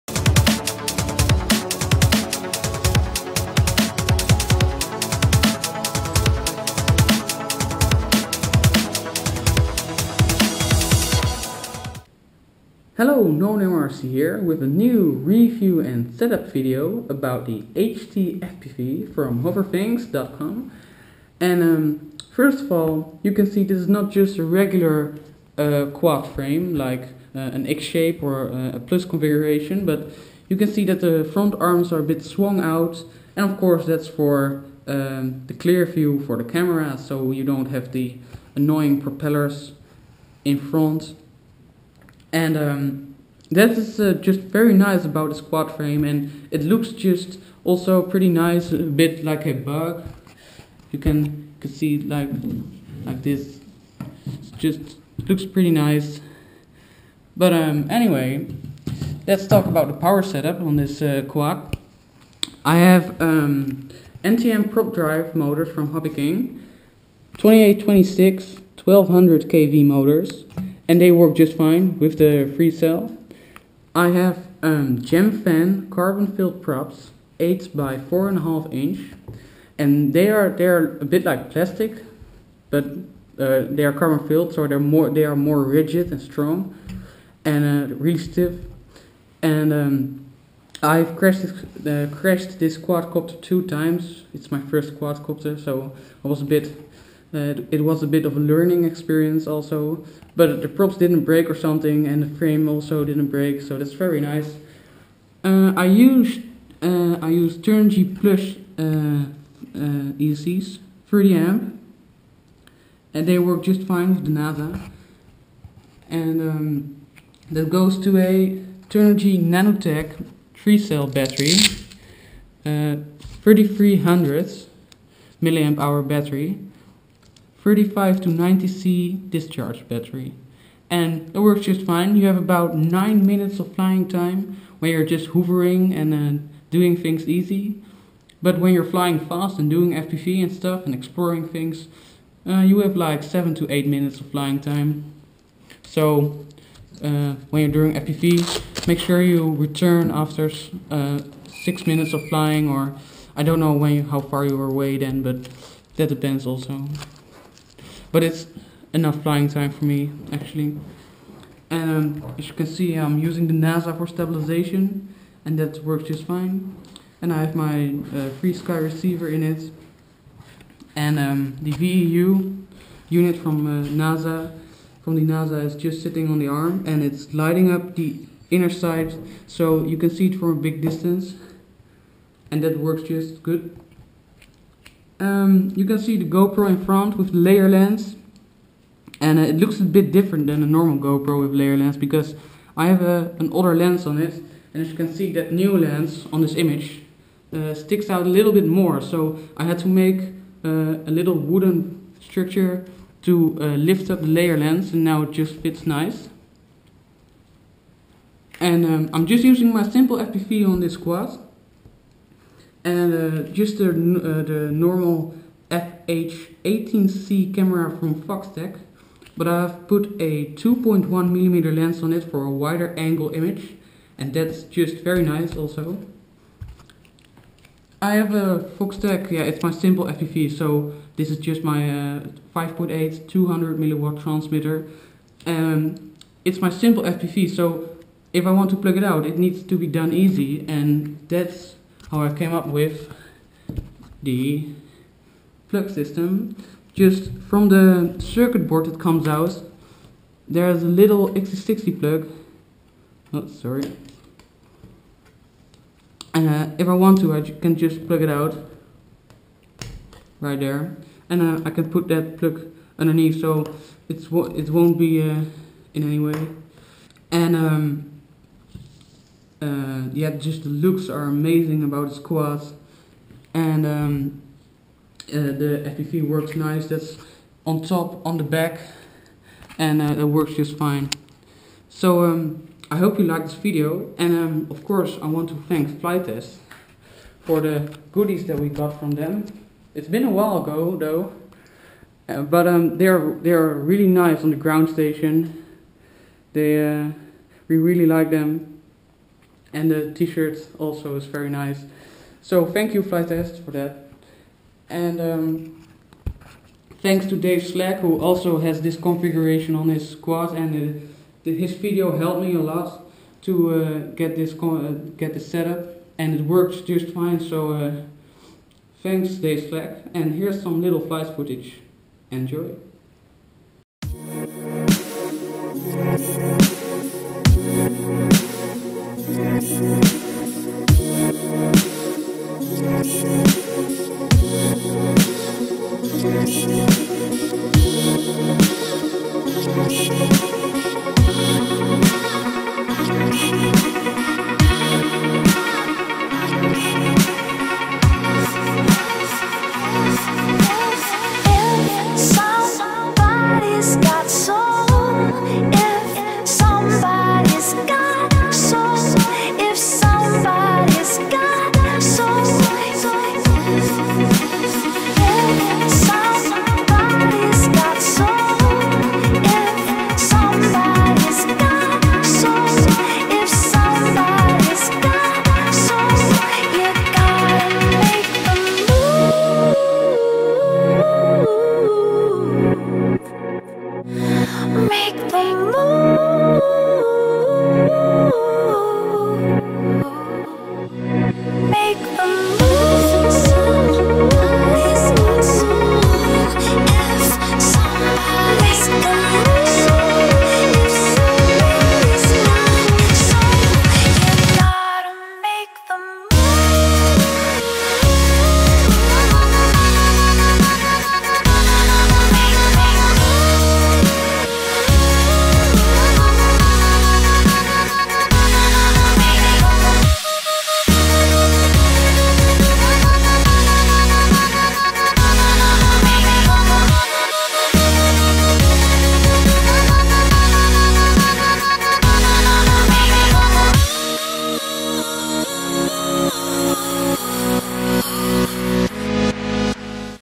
HT-FPV quadcopter review and setup -nonamerc-
This quad is perfect for FPV! It flies very stable so it's also capable for some aerial photography (except for the fact that it can only carry a gopro and not something like a Sony NEX-5). At the end of the video there's also some flight footage. Hope you guys like it!

These are not all the parts that I used, but these are the most basic things for the setup. You'll need to buy accessories like extra silicon wire, esc programming cards, prop adapters etc.

If you have any questions just ask them in the comment section :)